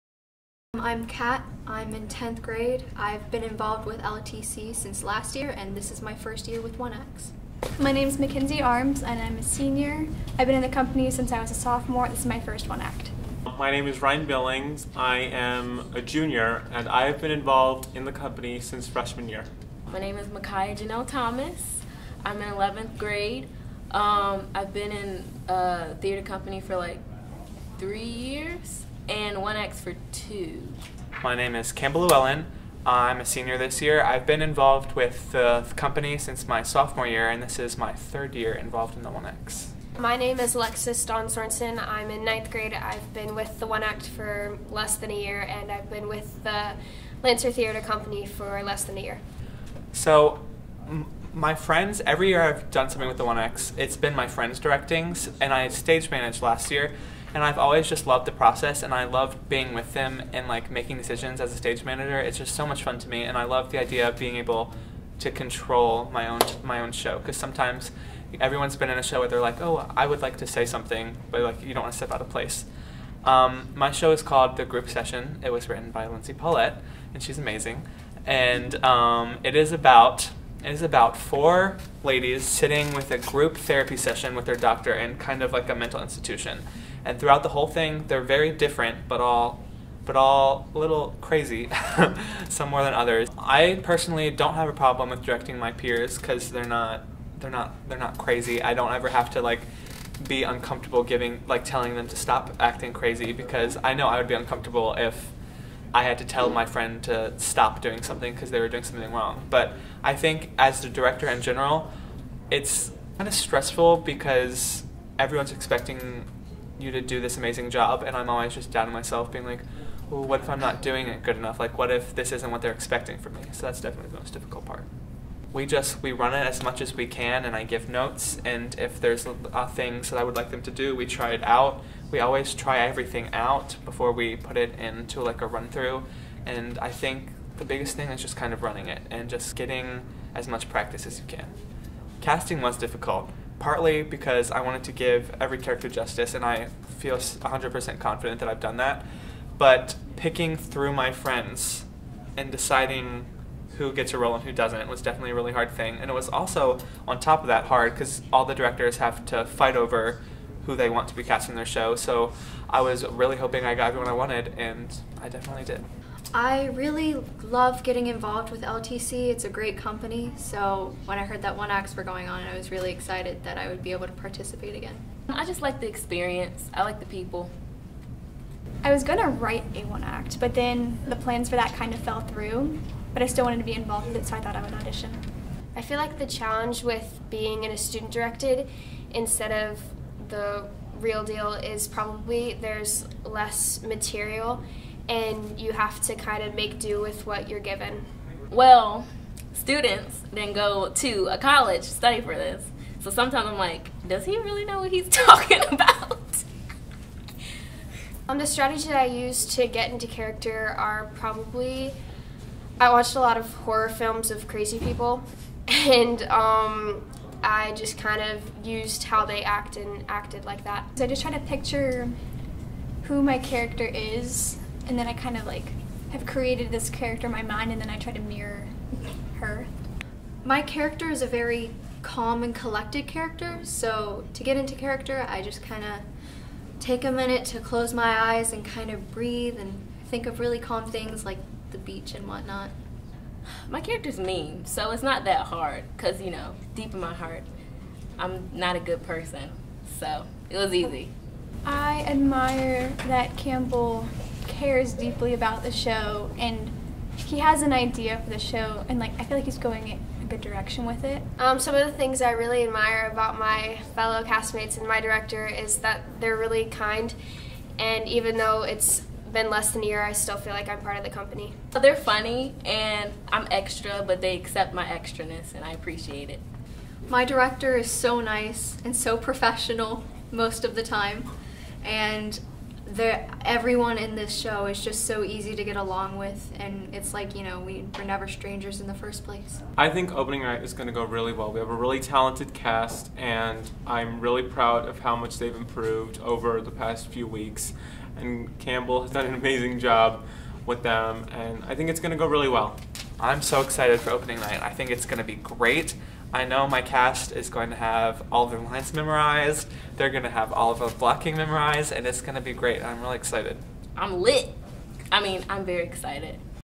Behind The Scenes of The Group Session
A behind the scenes look at the production of "The Group Session" ; a one act play written by Lindsey Paulette.

"The Group Session" is about four mental health patients living in a psychiatric hospital and their group therapy session with their doctor.

The show was directed by Campbell Luellen and stage managed by Ryan Billings.

The cast includes:
Miciah Thomas as Joanne
Kat Solberg as Doctor Miller
Mackenzie Armes as Meredith
Alex Sorenson as Sarah
Tala Castro as Caroline

A live performance of "The Group Session" will be held at Manchester High School on January 17th and 18th of 2019. For more information about purchasing tickets or about the Lancer Theatre Company in general go